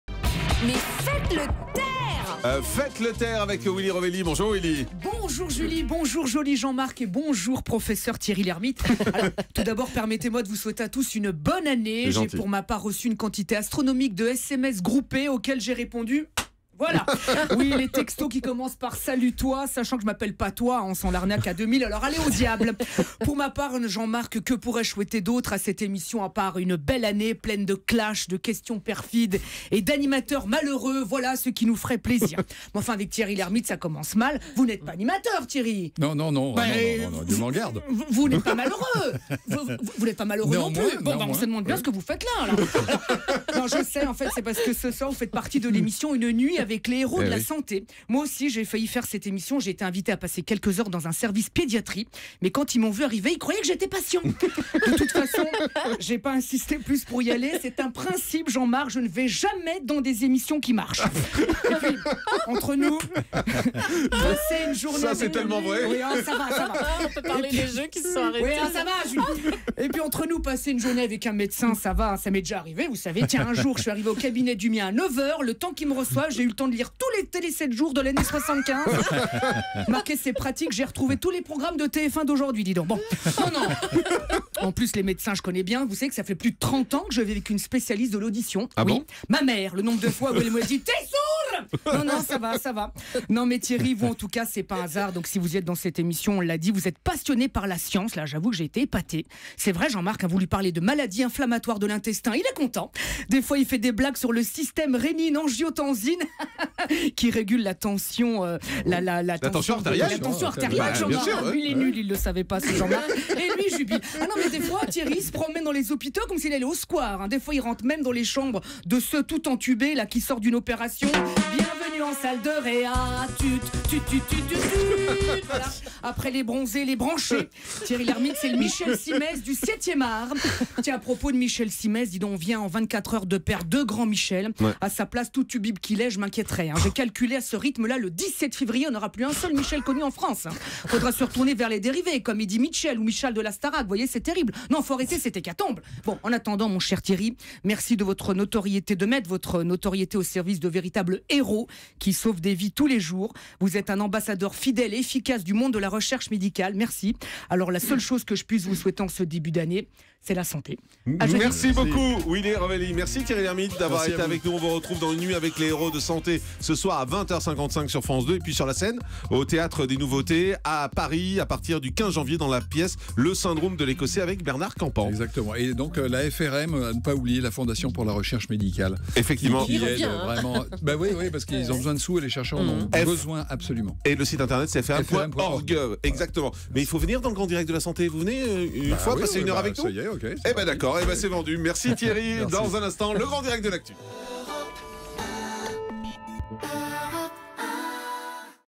Bonne santé pour 2016 ! - Chronique humour de Willy Rovelli
Willy Rovelli n'a pas voulu participer à l'émission Une nuit avec les héros de la santé car il a pour principe de ne jamais participer à une émission qui fait de l'audience.